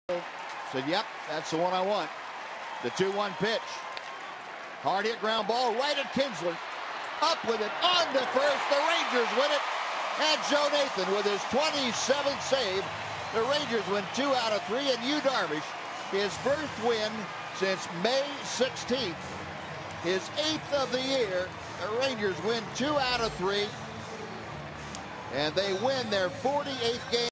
CIN@TEX: Nathan gets groundout to seal the victory
6/30/13: Joe Nathan gets Shin-Soo Choo to ground out softly to close out a 3-2 Rangers' win and earn his 27th save